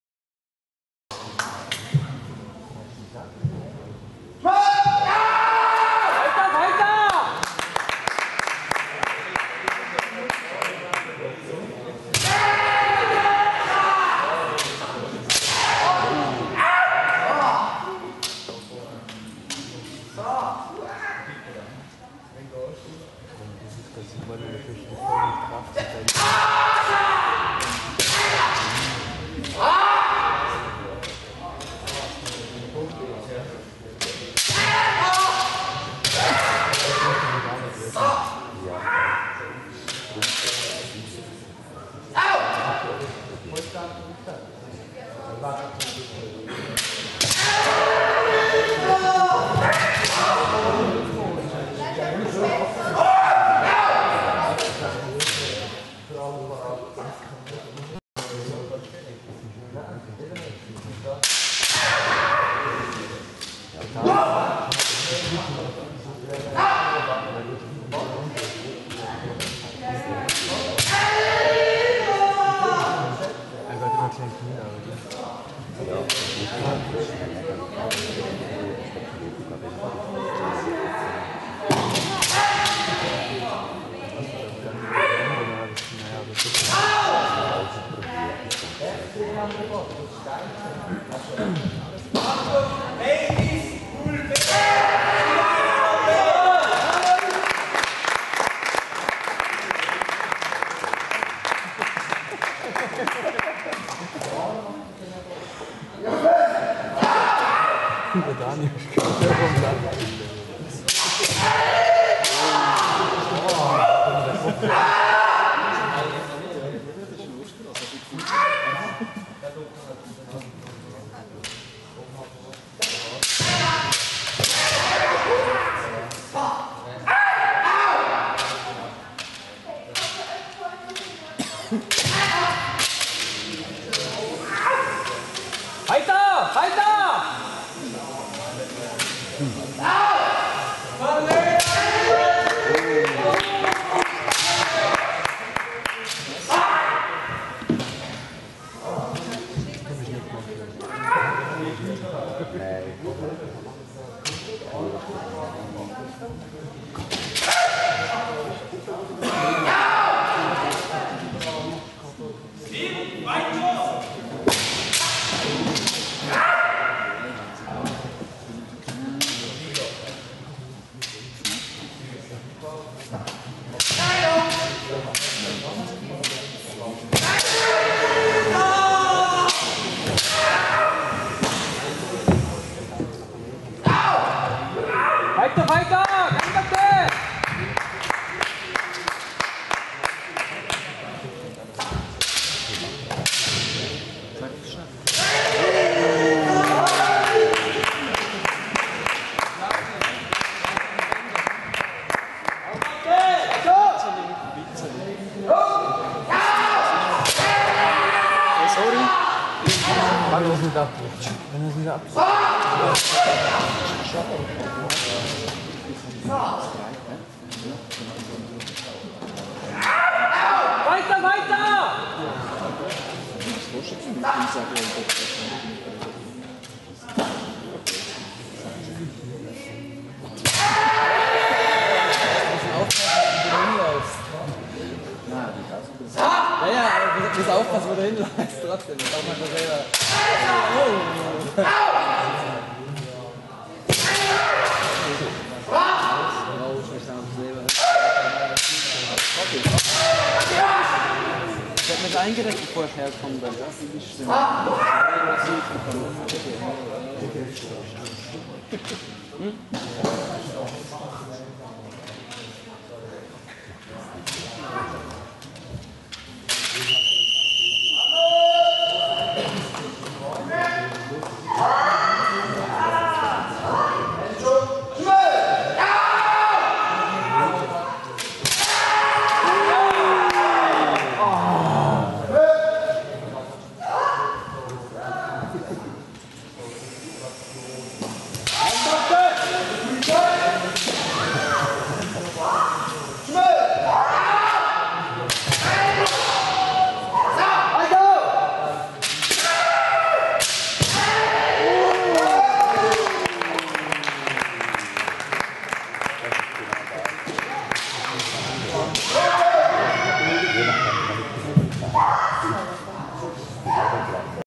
Basilisk 2011 : D. Pipola - S. Roth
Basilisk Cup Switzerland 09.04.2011
white: D. Pipola, Tshiku Sei Kan Kendo Club Basel
red: S. Roth, Tshiku Sei Kan Kendo Club Basel